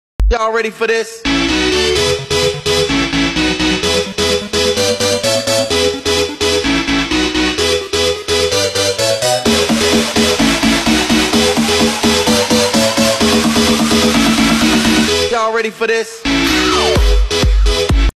Halifax Panthers 150th anniversary season
Celebrating Halifax Panthers 150th anniversary season FAX150 by turning the classic blue and white hoops into a shot! 🔵⚪️

Thank you all for your continued support, Go A-Team!!